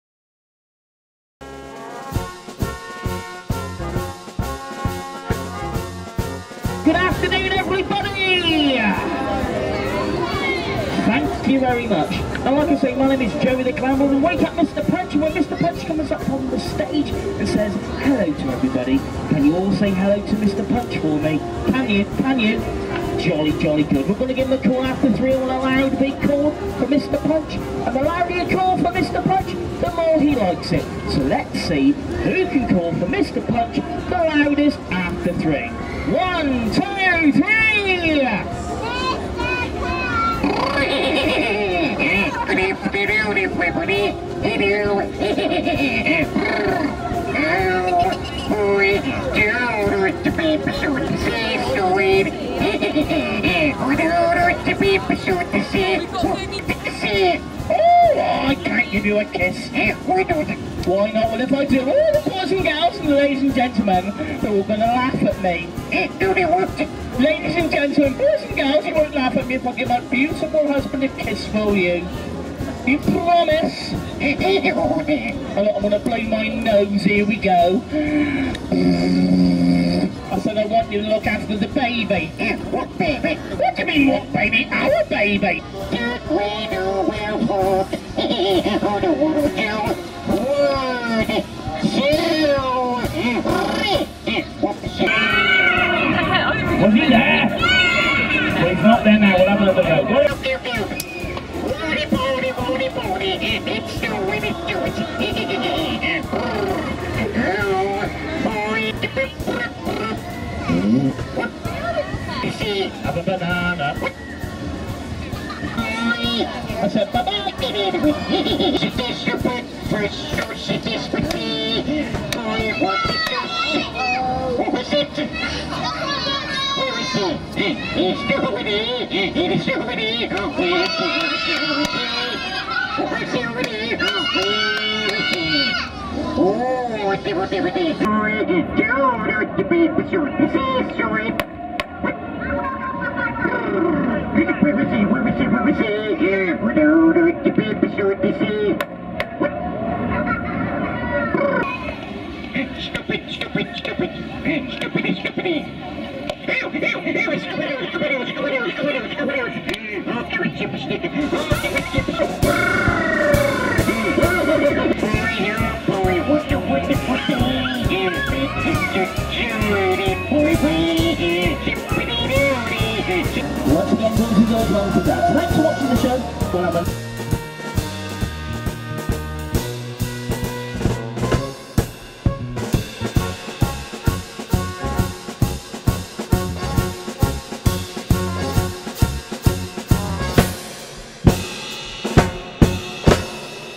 PUNCH & JUDY. Paul Wheeler
Punch & Judy excerpts by Professor Paul Wheeler from Bristol